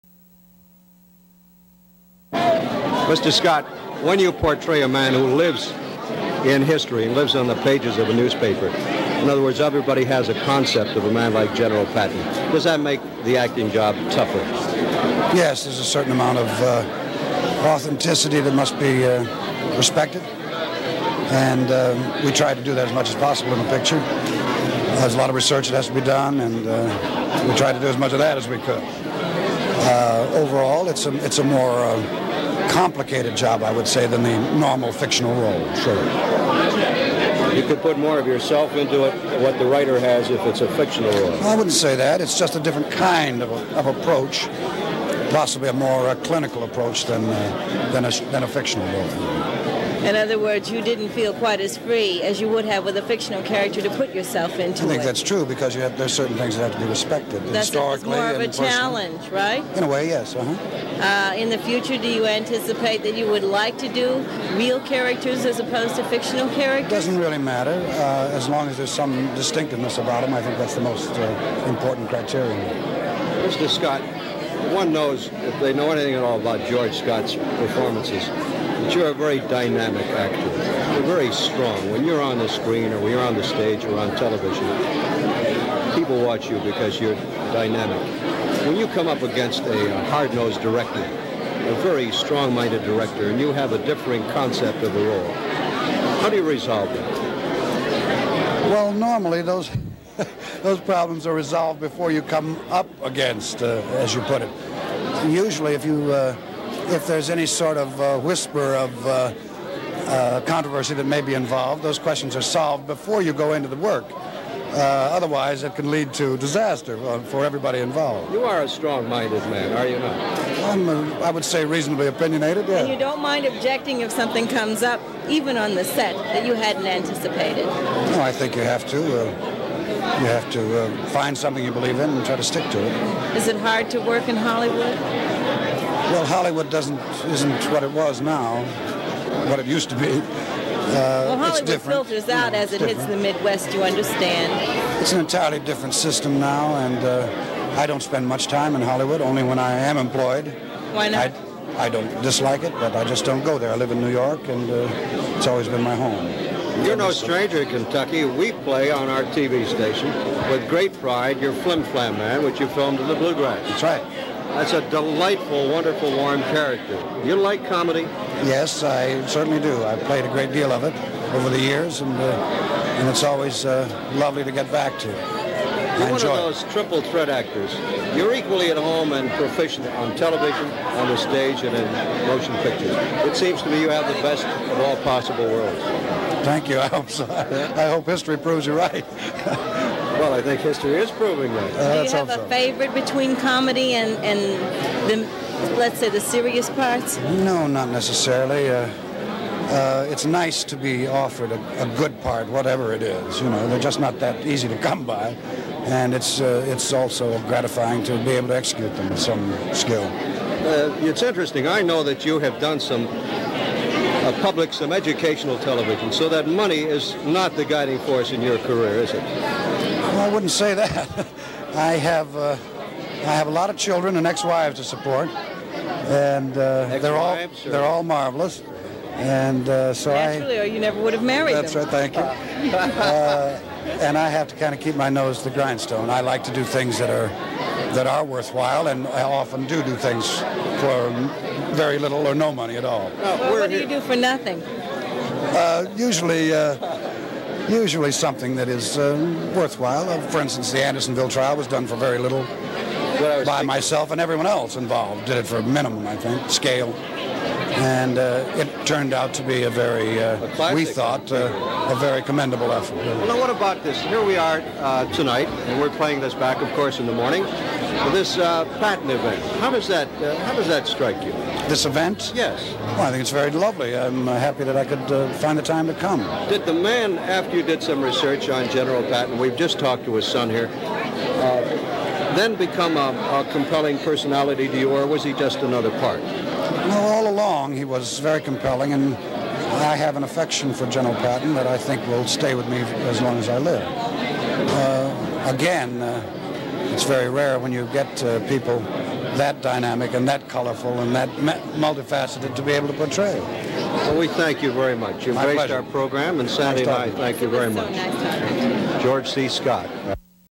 Actor George C Scott
Storied actor who starred as Gen. George S Patton.
Interviewed by Metz and Sandye Bender